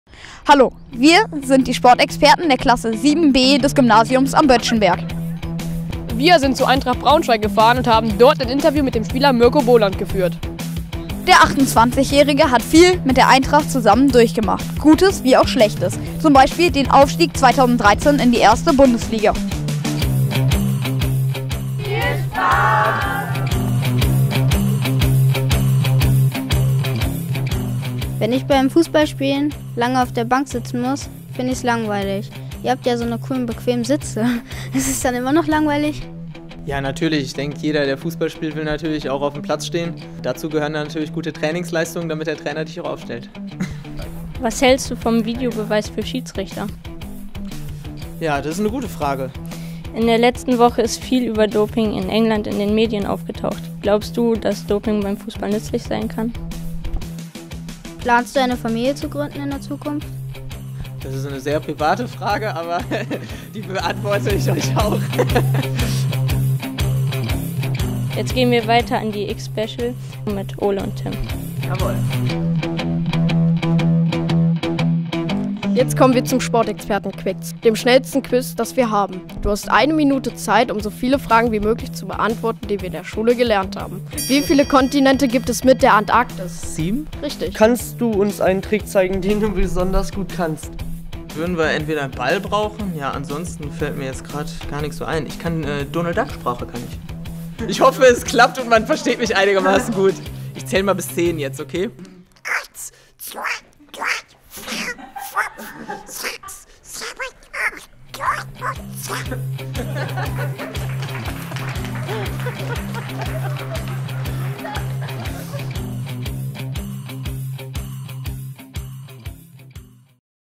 Braunschweigs Publikumsliebling Mirko Boland [Trailer]
Der Mittelfeldmotor überrascht die Helmstedter SportXperten in bester Laune.
Da beim Interview kein Ball zur Verfügung stand, überzeugte er bei den SportXperten-TriX mit fabelhafter Donald Duck Stimme.
Super Trailer-unbedingt ansehen.